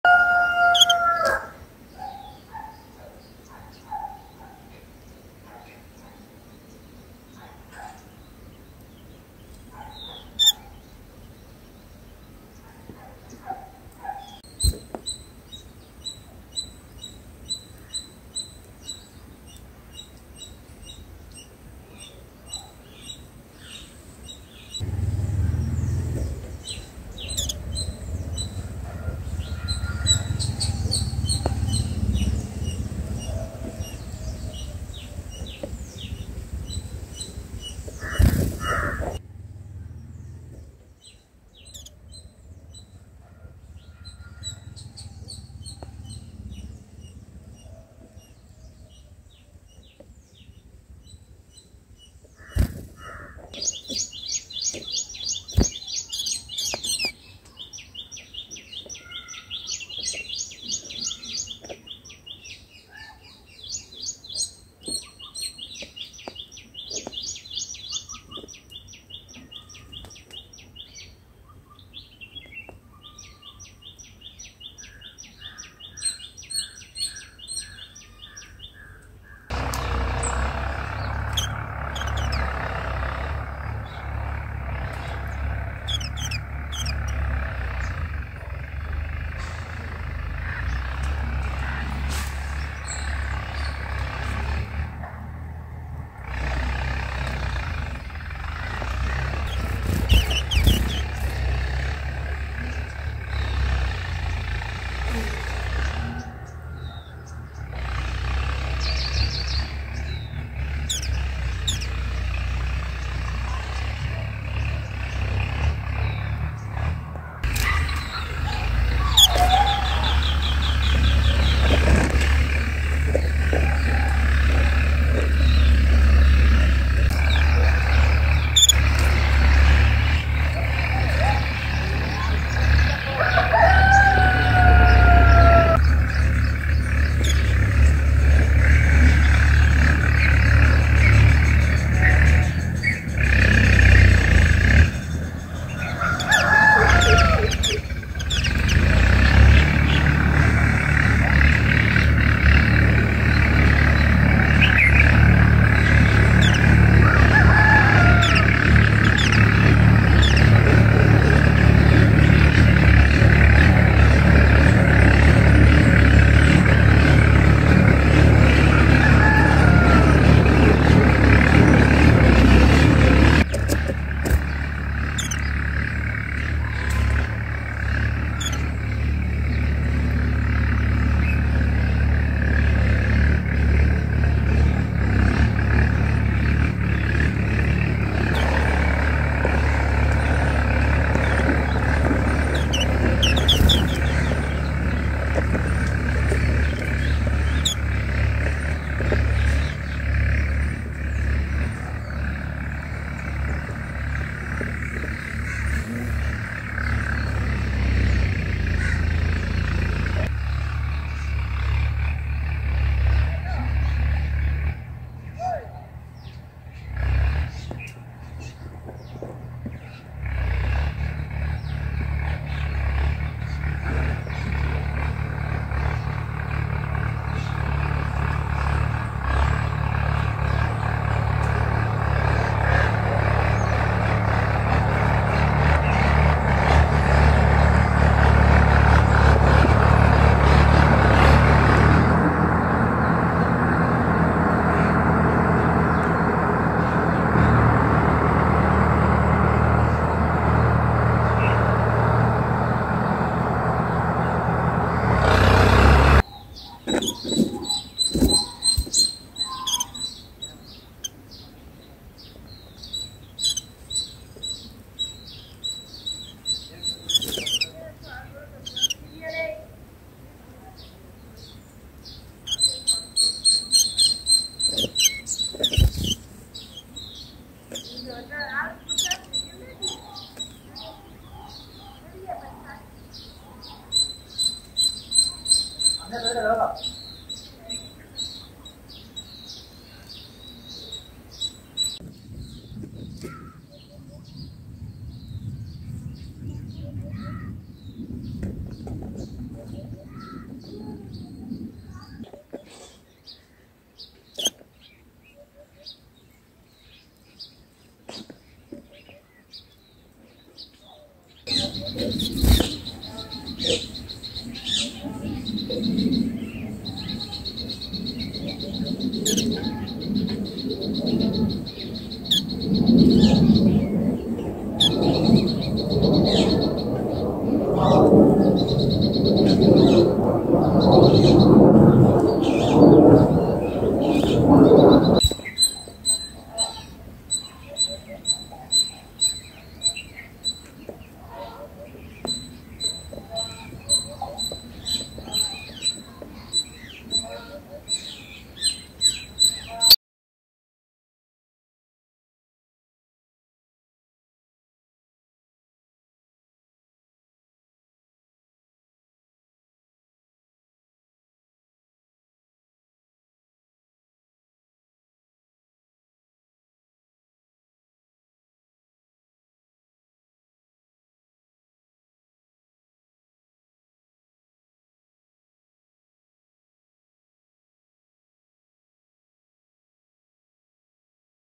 White Eye feeding PIG WORM to baby birds in nest ||| White eye baby birds video in nest | day2 prt1
To Get AI based Tips to Grow your YouTube Channel for free Signup

Low Cost & Best Set Up we Use visit:-
In INDIA
In USA